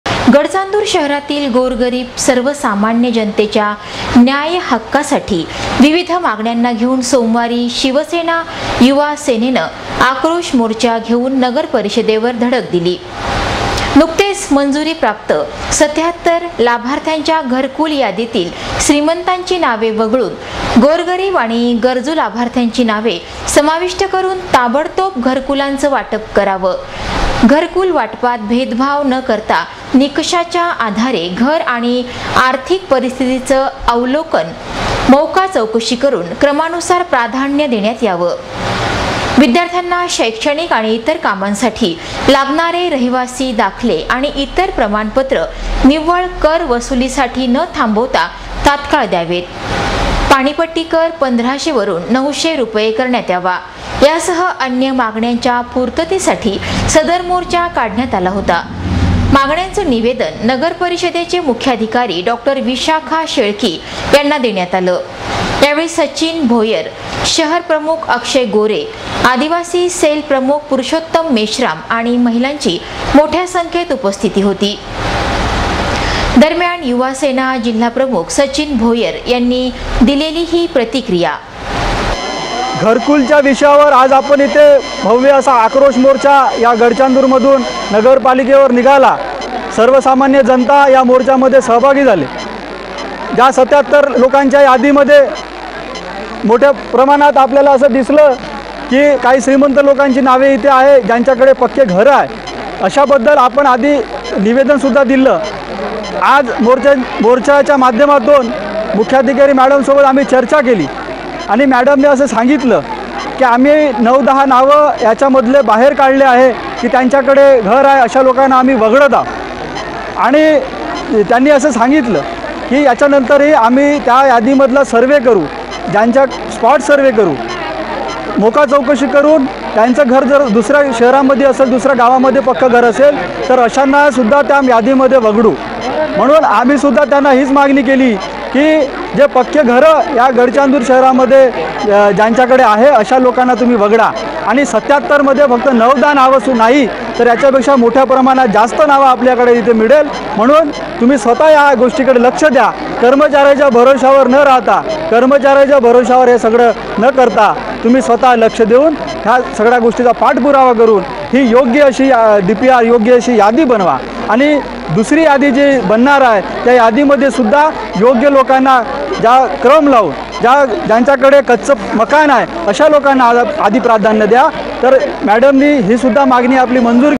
*गडचांदूर चंद्रपूर*- गडचांदूर नगरपरिषदेवर विविध मागण्यांसाठी धडकला आक्रोश मोर्चा
*गडचांदूर चंद्रपूर*-  गडचांदूर नगरपरिषदेवर विविध मागण्यांसाठी धडकला आक्रोश मोर्चा - घरकुलासह इतर मागण्या ऐरणीवर- महिलांची लक्षणीय उपस्थिती